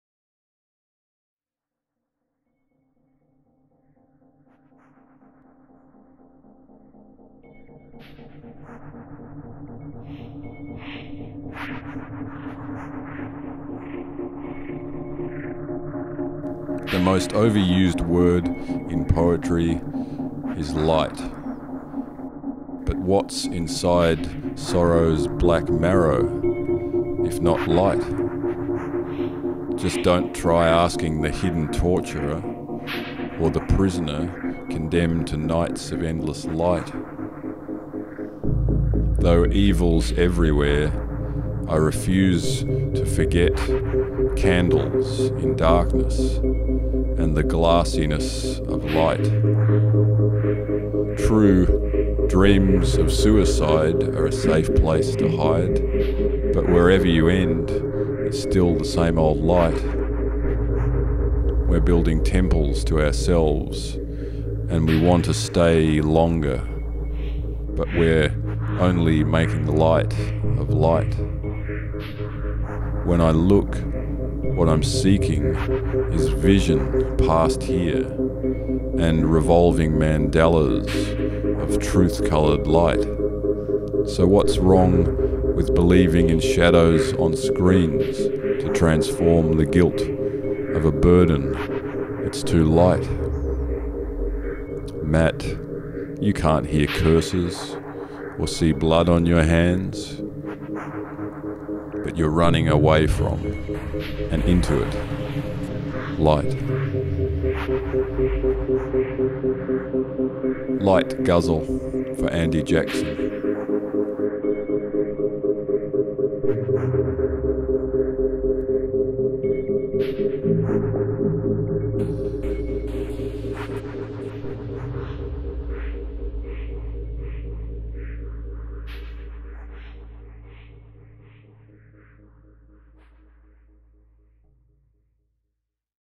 Matt Hetherington & Marie Craven - Light Ghazal
Video poem collaboration between poet Matt Hetherington and film-maker Marie Craven. Music by Masonik. Source footage from Mono No Aware.